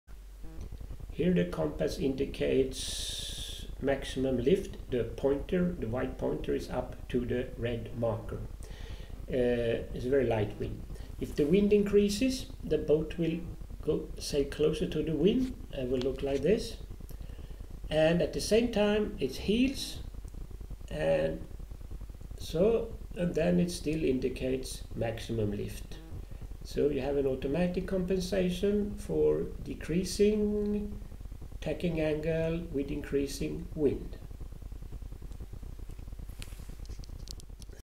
Automatic compensation for decreasing tacking angle in more wind. Ansar Tactical Compass.
About the Ansar Compasses World's most expensive tactical compass.  Non electronic. An Ansar Compass.